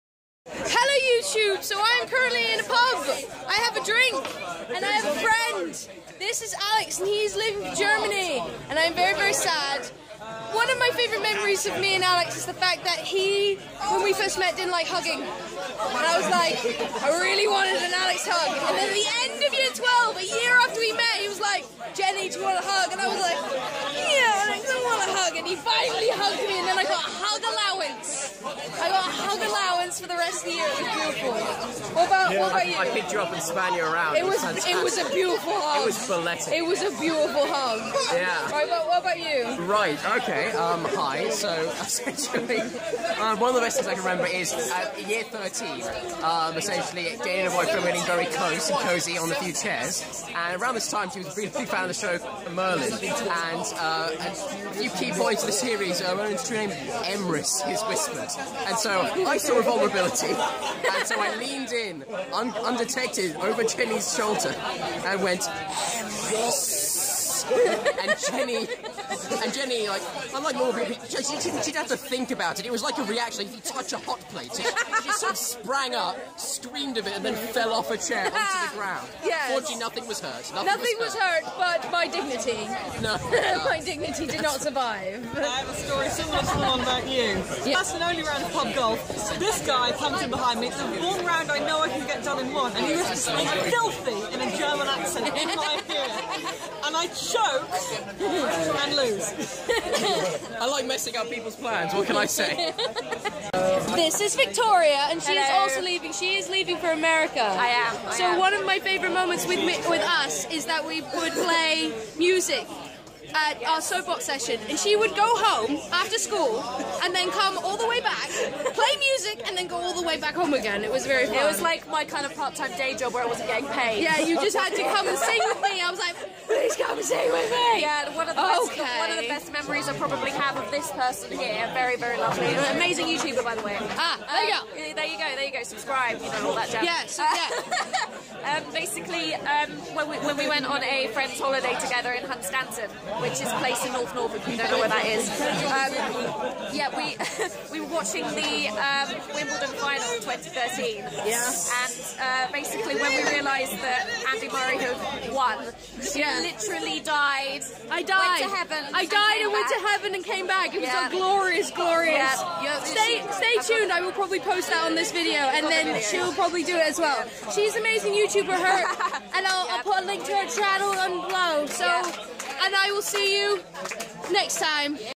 A Slightly Drunken Vlog
VEDA Day 29:
A slightly drunken me says goodbye to two of my closest friends who are going abroad for a year. I gonna miss you both.